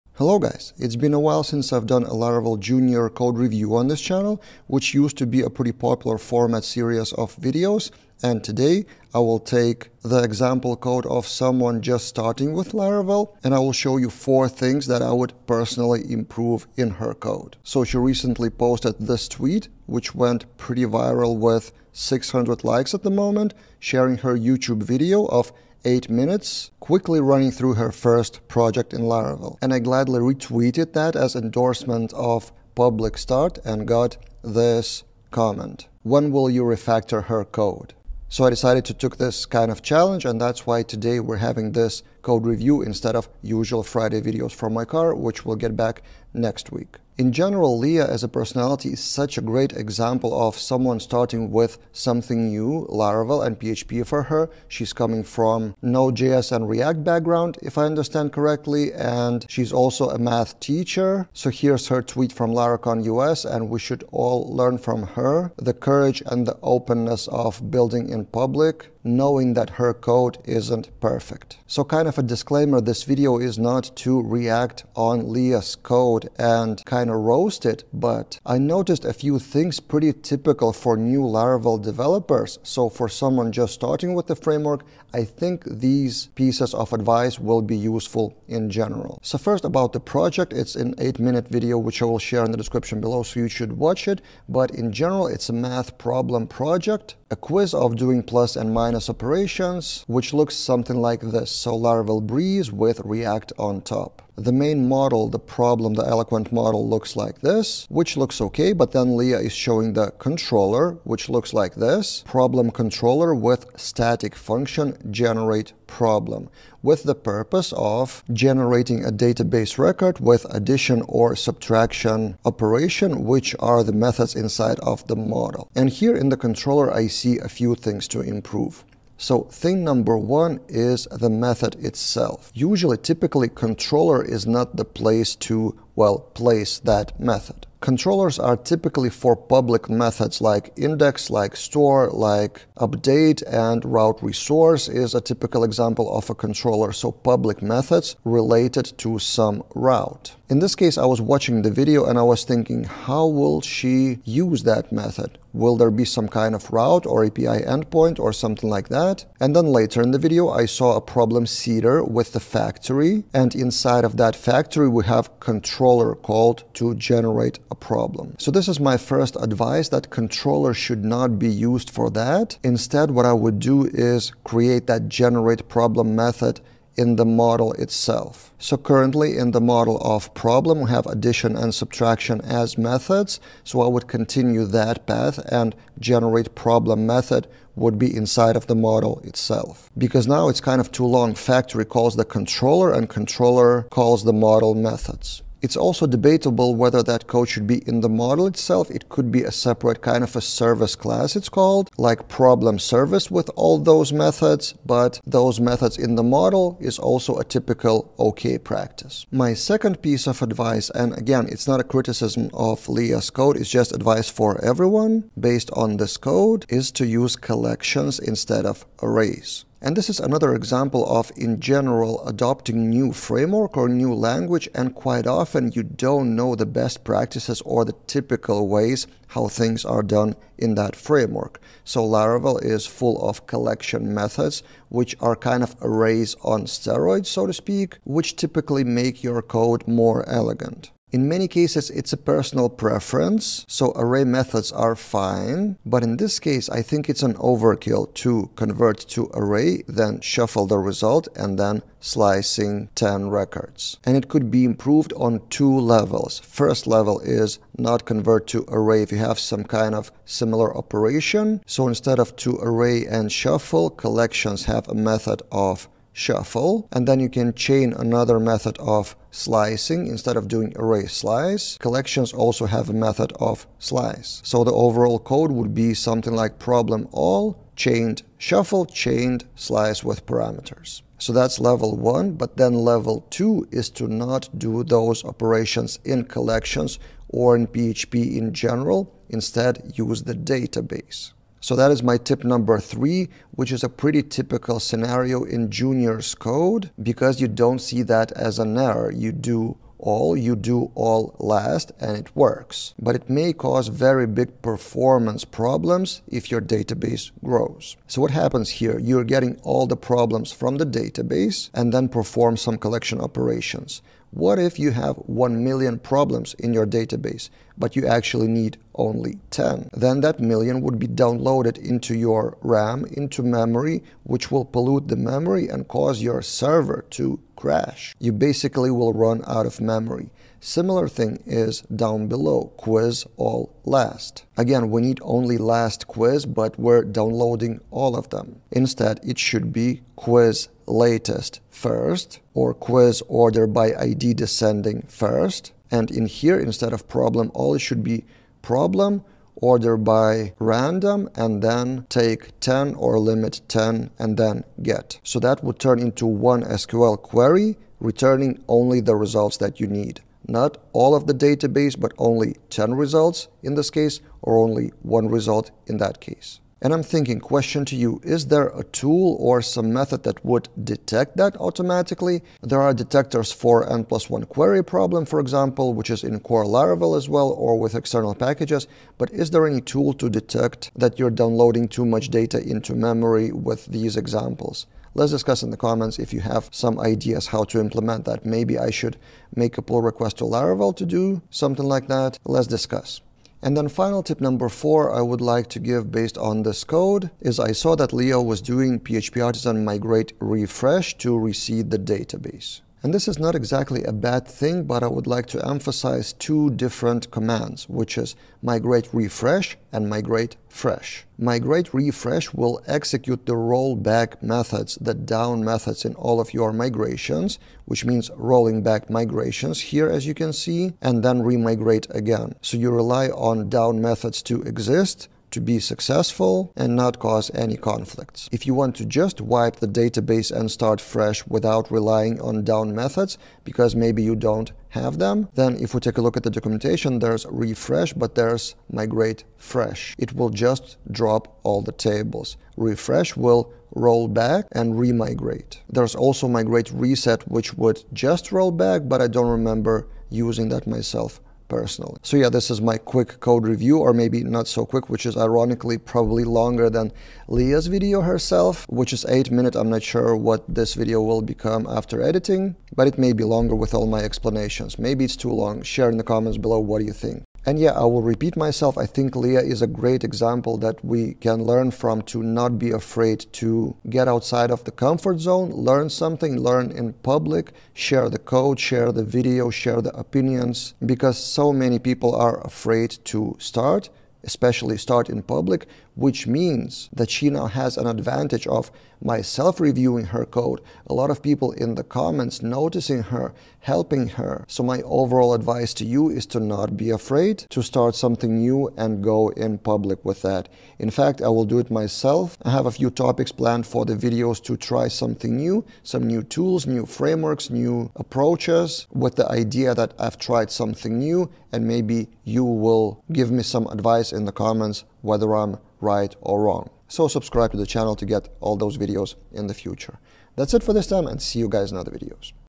4 Tips to Improve Leah's Laravel Code
I saw a great candidate for a quick code review, so, here we go!